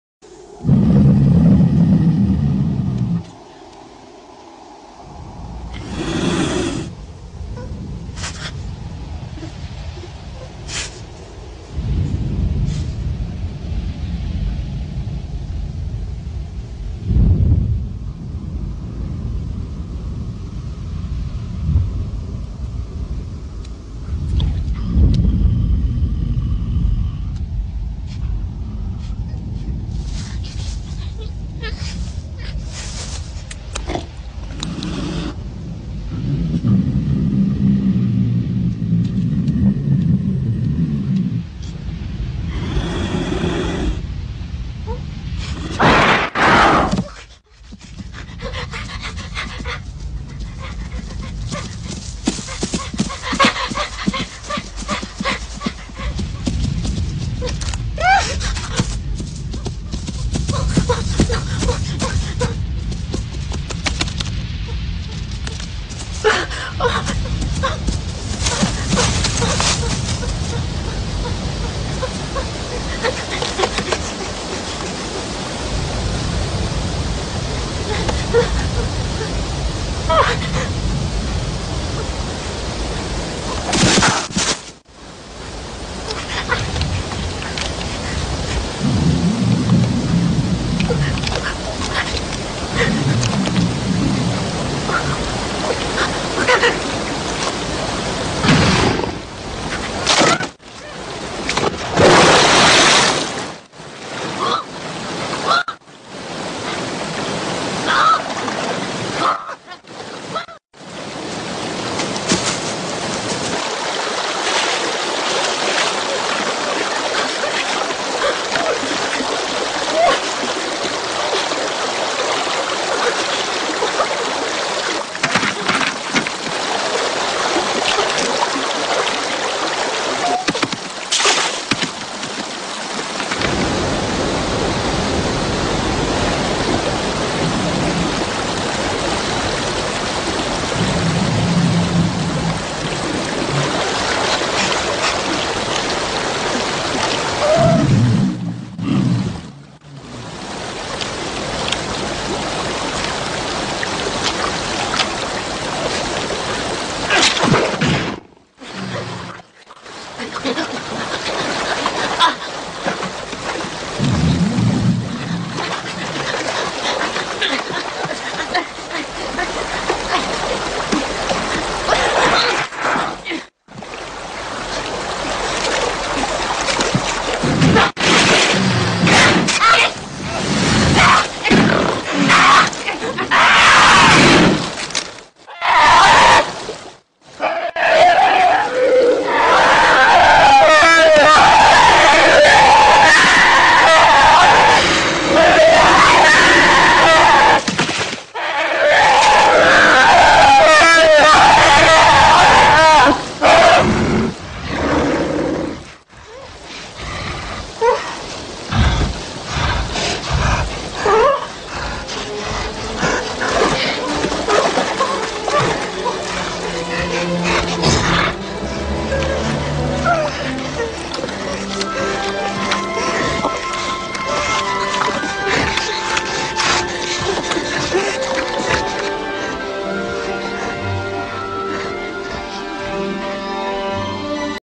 Пума против медвежонка
Иллюстрация к проповеди "Сила нашей победы"